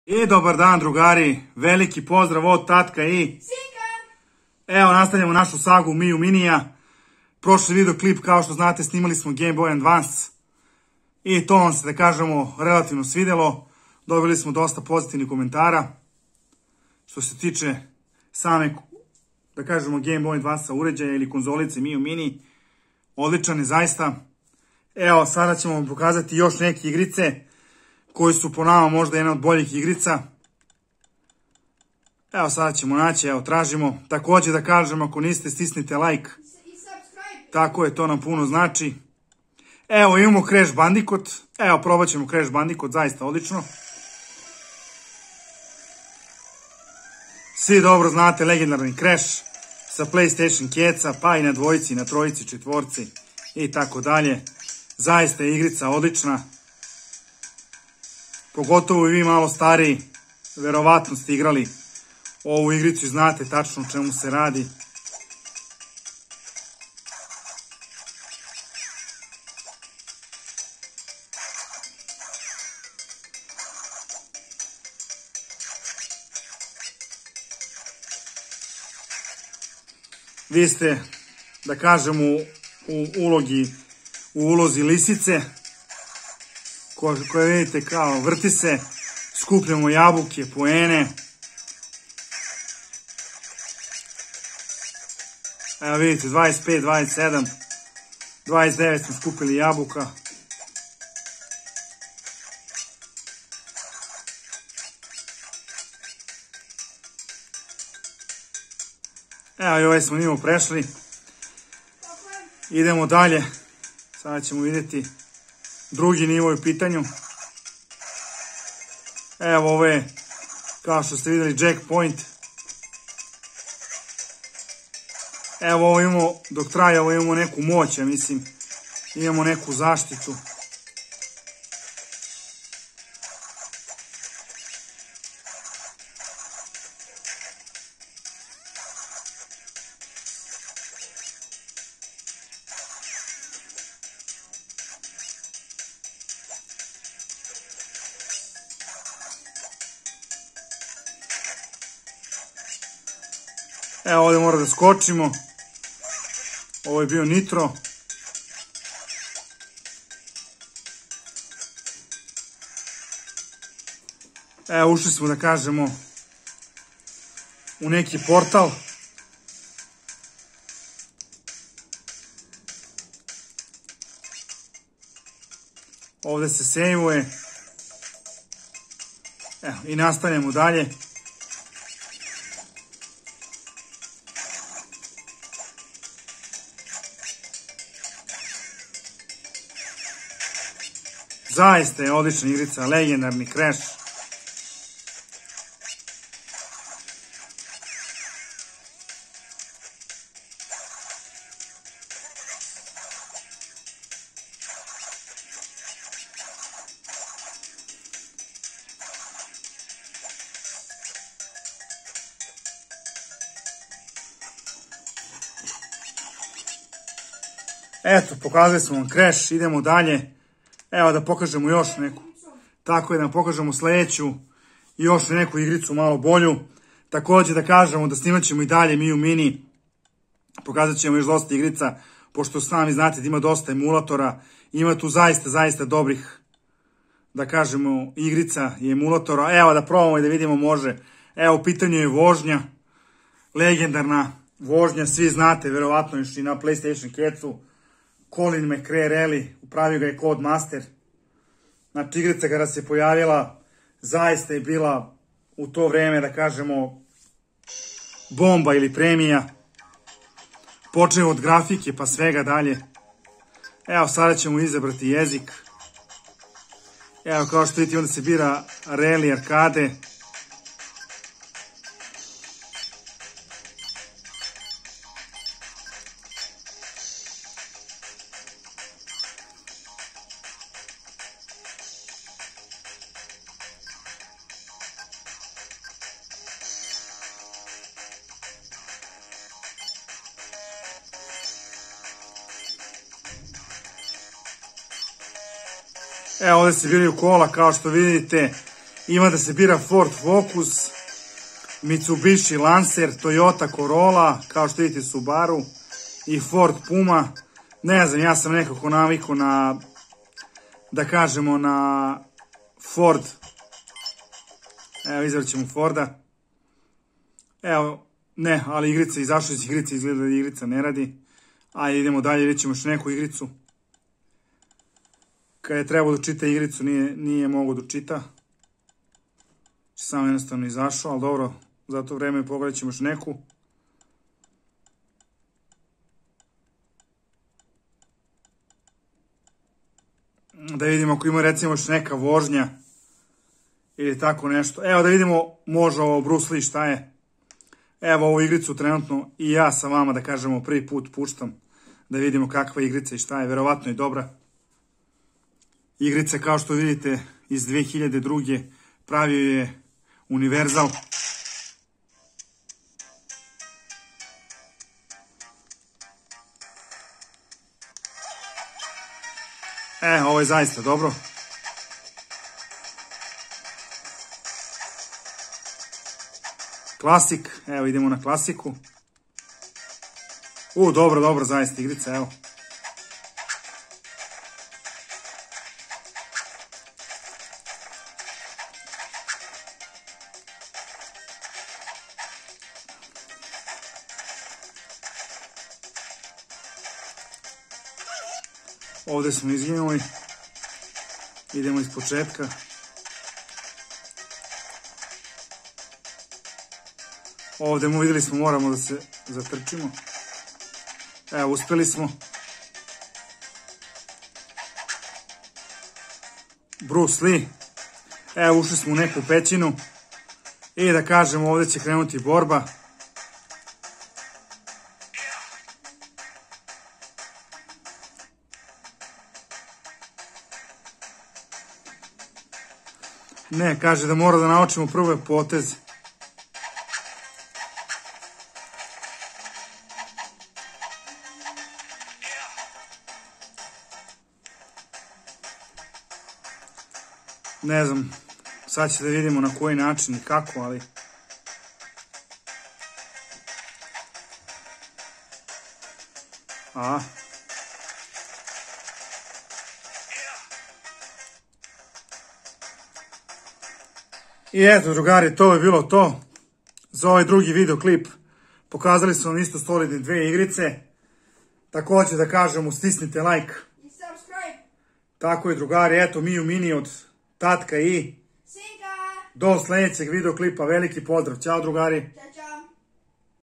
Miyoo mini gba🔥🔥🔥
Miyoo mini odlicna konzola od Tatka i Sinka gba emulator.